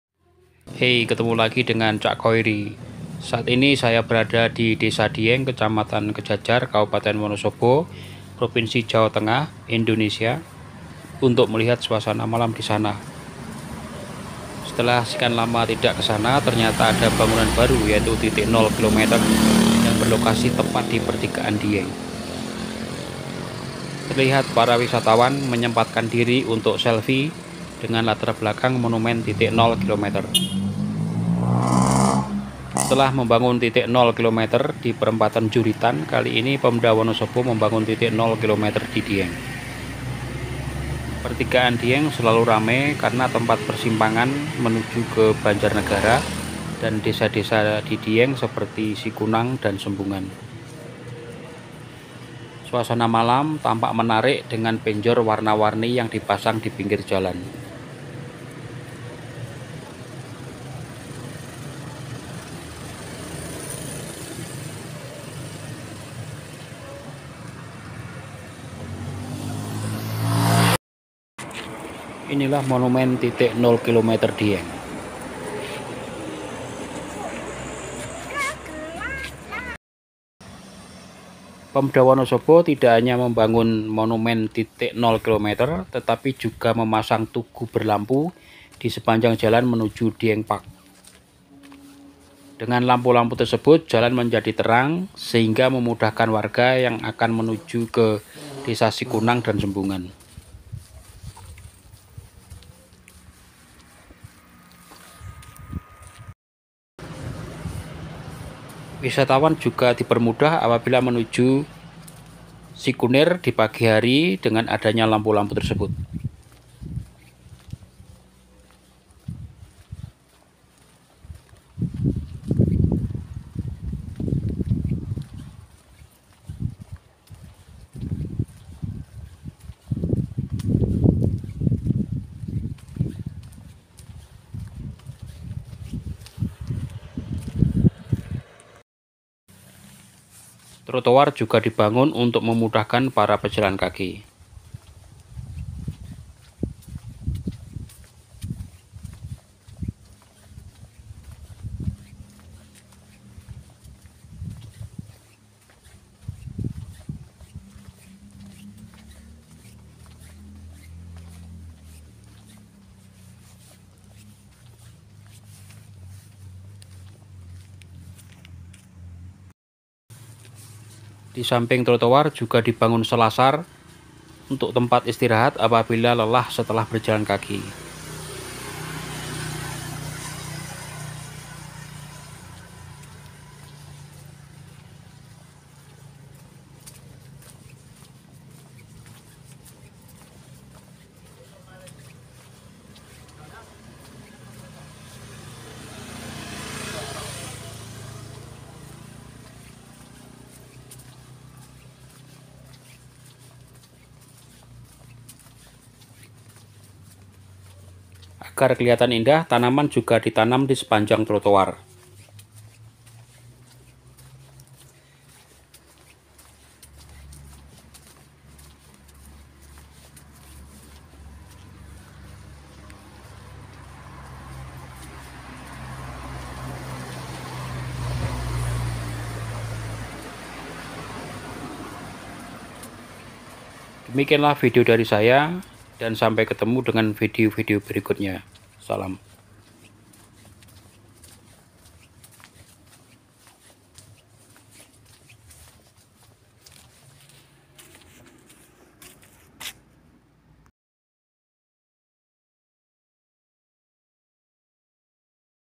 Monumen Titik Nol Kilometer, Dieng, Wonosobo.
Monumen Titik Nol Kilometer berlokasi di desa Dieng, Kec.Kejajar, Kab.Wonosobo, Prop.Jawa Tengah, Indonesia.